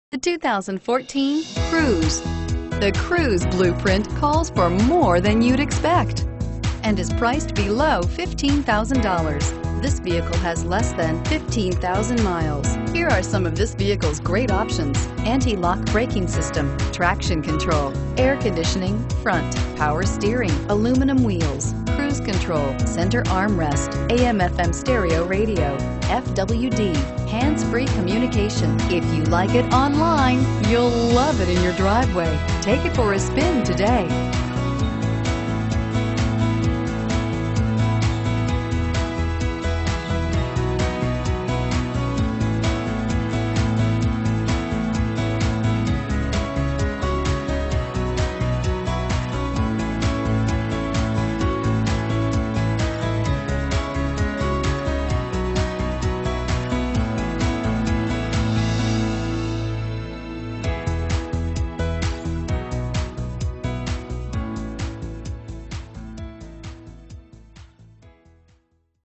2014 Chevrolet Cruze Cold Spring, St Cloud, Willmar, Alexandria, MN 5076XX
ONE OWNER - LT PACKAGE - 38 MPG HIGHWAY - USB PORT - BLUETOOTH - SUPER CLEAN - LIKE NEW - BUMPER TO BUMPER WARRANTY - HUGE VALUE - PREMIUM CLOTH SEATS - This Cruze is a huge value for anybody looking to save some money on gas. It has super low miles and needs nothing but a new home. This Cruze is priced to move and will sell fast.  This vehicle is currently at our COLD SPRING CHEVROLET location at 1050 East Highway 23 fifteen miles west of St. Cloud Minnesota.  Please call to verify as we transfer vehicles to and from our Willmar location.